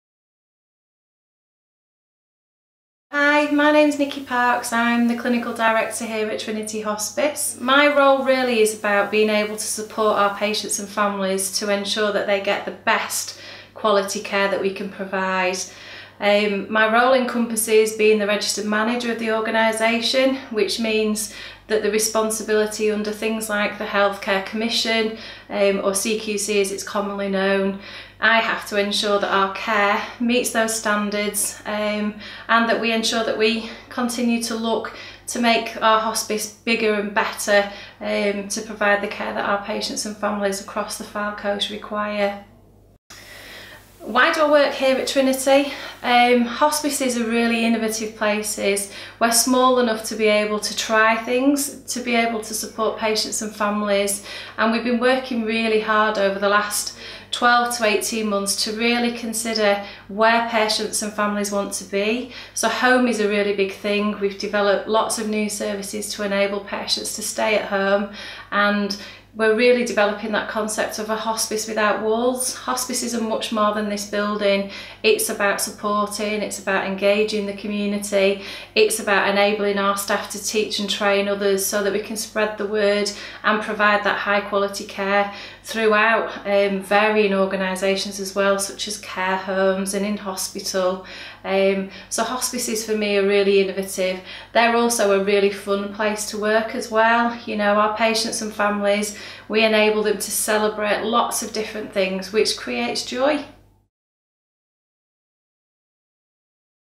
Hospice Care Week 2018 - Nicky Parkes
It's Hospice Care Week 2018! Everyone who works at Trinity has their own story of why they work here and what difference we make for patients. Watch our Clinical Director, Nicky Parkes' video here.